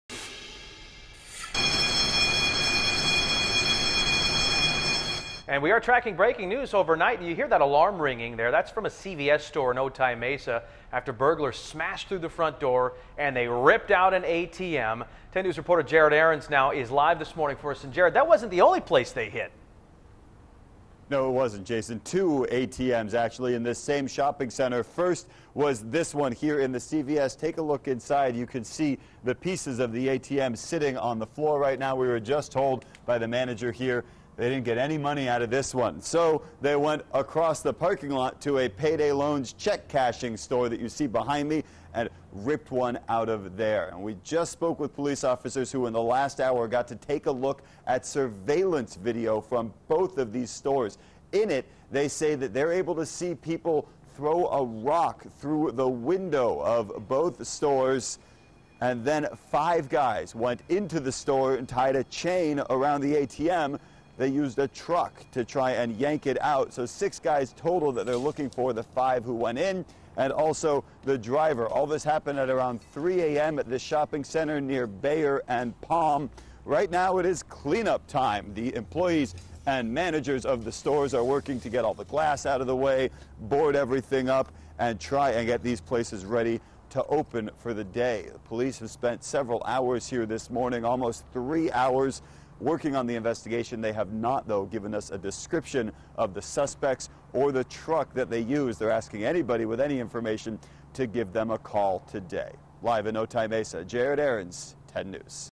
2 ATMs ripped from Otay Mesa strip mall
Burglars ripped out two ATMs from two separate Otay Mesa businesses overnight Monday.